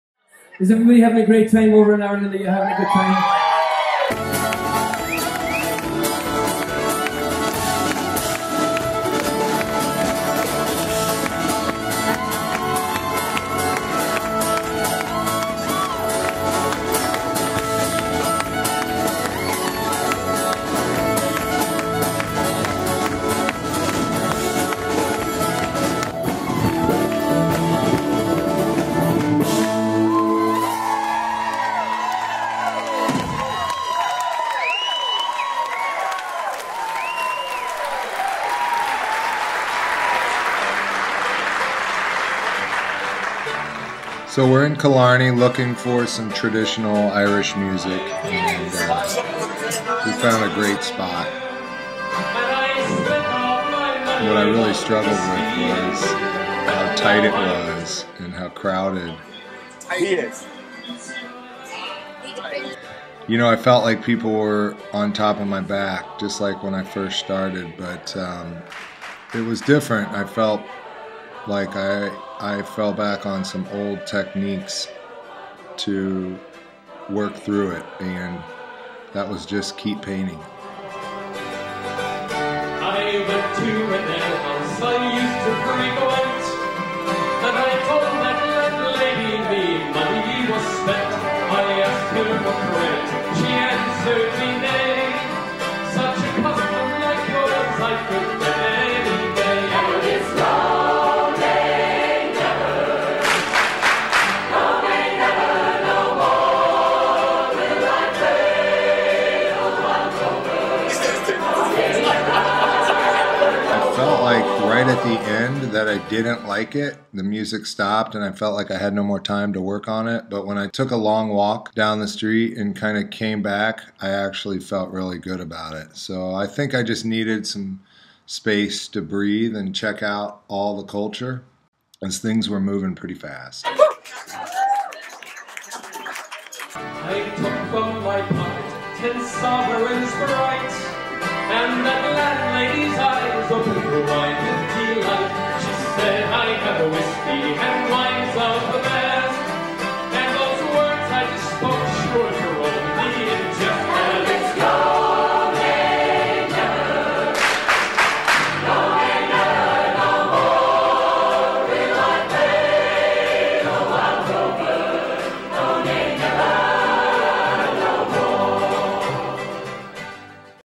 Ireland's 33 - Crowded Pub, Killarney, County Kerry - Painting 7
John finds himself squished in a local bar in Killarney, but the music drives him to paint yet another classic Irish scene.

To see the paintings LIVE:
KANSAS CITY Bukaty Pop-up Gallery at 3161 Wyandotte St., Kansas City, MO. 64111  Thursday March 3rd at 6pm